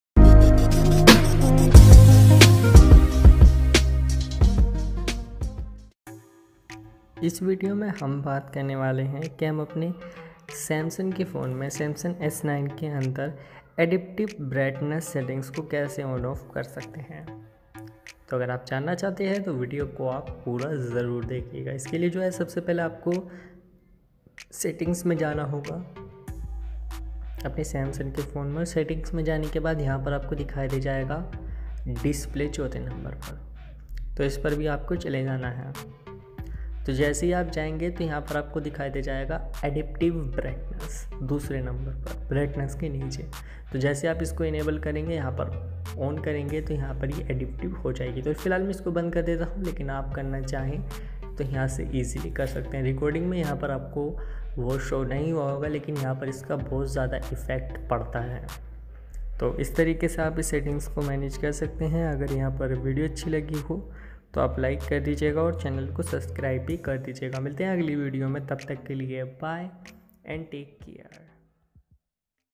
How To On Off Adaptive Brightness Settings in Samsung galaxy s9 Mobile
Is video Mai baat ki hai adaptive settings Keep track of your brightness adjustment and apply them Automatically in similar lighting conditions ke bare Mai